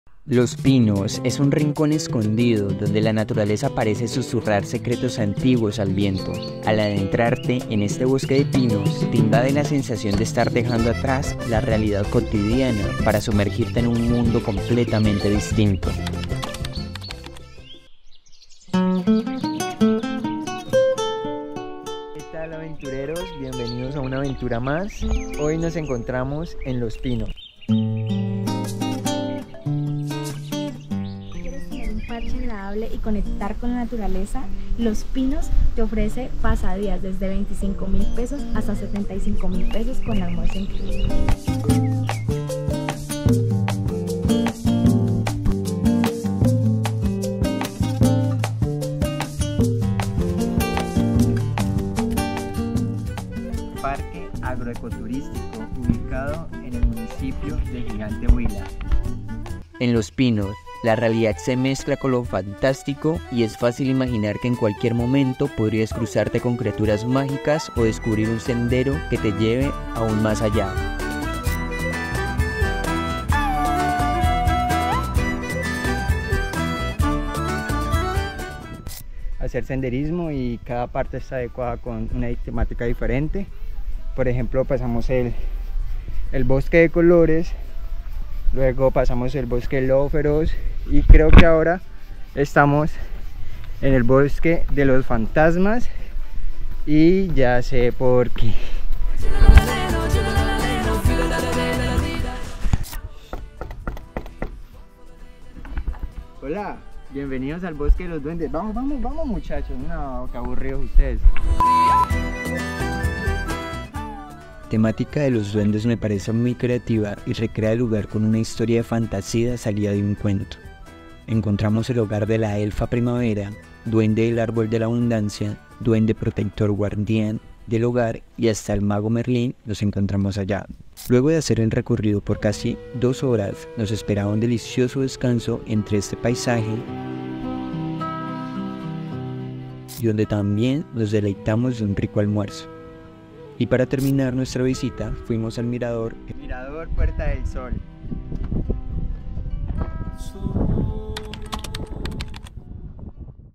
Parque Agroecoturistico | Gigante Huila | Bosque de Pinos 🌲 | Wil Calderón
Te llevaremos a un recorrido por el Parque Agroecoturístico en Huila, donde encontrarás un hermoso bosque de pinos. ¡Acompáñanos en esta aventura!